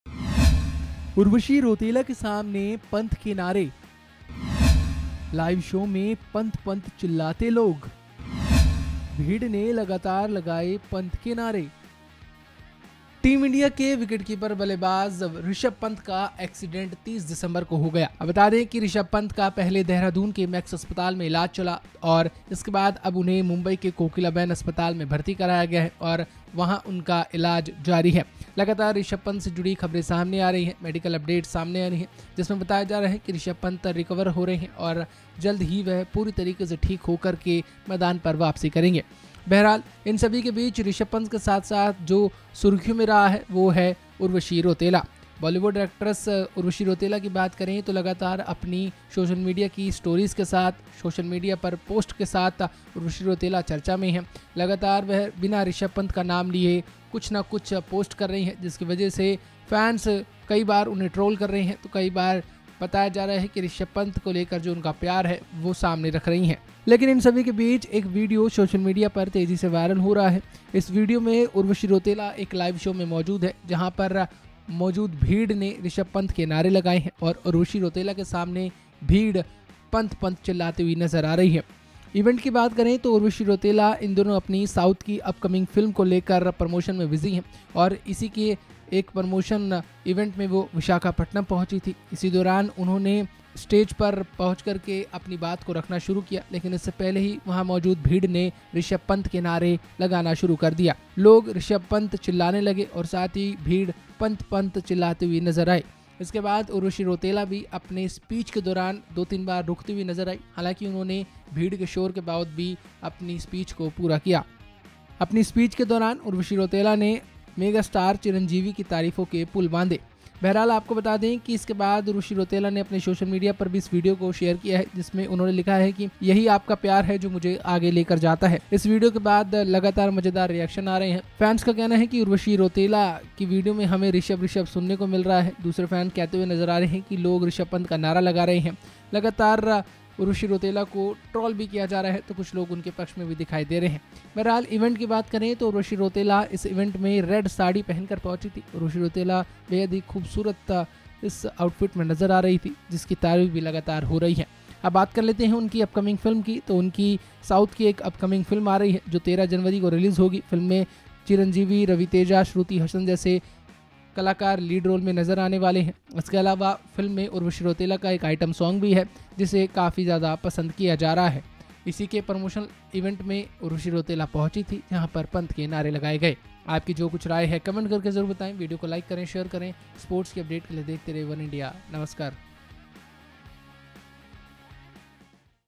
People Raised slogans of Rishabh Pant in Urvashi Rautela's live Show, Watch Video | Oneindia Sports
उर्वशी रौतेला (Urvashi Rautela) फिर ट्रोल हो रही हैं. प्रमोशनल इवेंट (Film Promotional Event) में वे जैसे ही स्टेज पर आती हैं, लोग ऋषभ पंत (Rishabh Pant) चिल्लाने लगते हैं. लोगों के जोर- जोर से पंत- पंत (People Shounting Pant Pant)  चिल्लाने की वजह से उर्वशी अपनी स्पीच देते हुए बार-बार रुक (Urvashi Rautela on Rishabh Pant) जाती हैं. उर्वशी अपनी स्पीच दे रही हैं और लोग ऋषभ के नारे लगाना बंद नहीं कर रहे हैं.